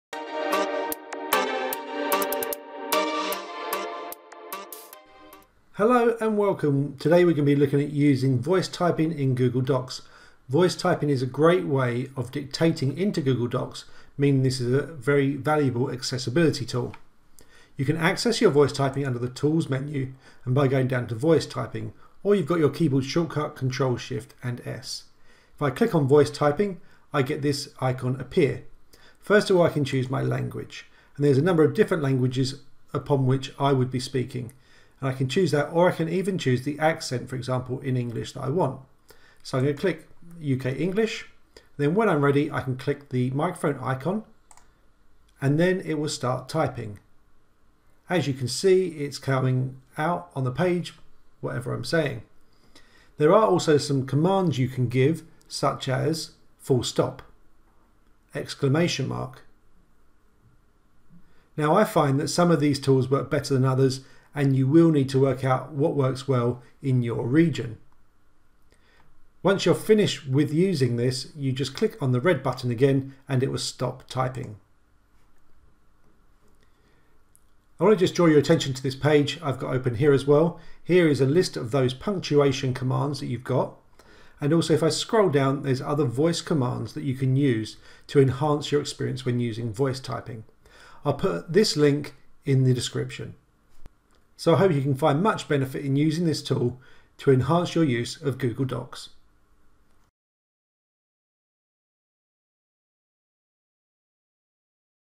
Voice typing in Google Docs
The ability to use voice typing in Google Docs is a great way to dictate directly into your document. This is a valuable accessibility and time saving tool.